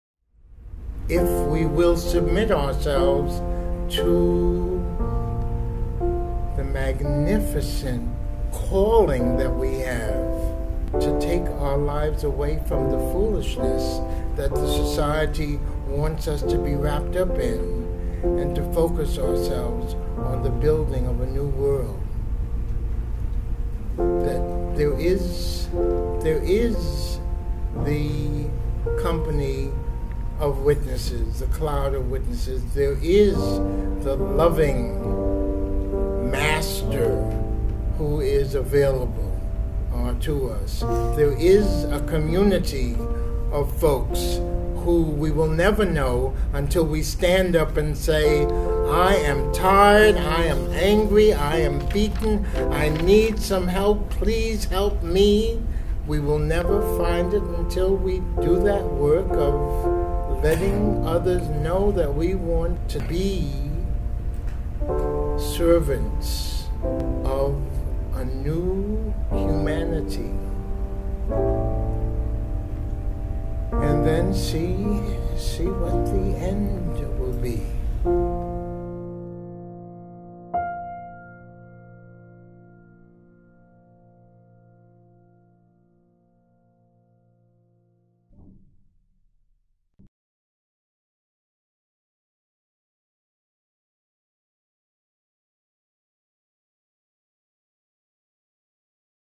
A Tribute to Vincent Harding
A tribute I put together in memory of Vincent Harding (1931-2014), a long-time friend of Sojourners, a historian, and a scholar. The video was played at the Summit, Sojourners' annual leadership gathering for faith-based social justice voices.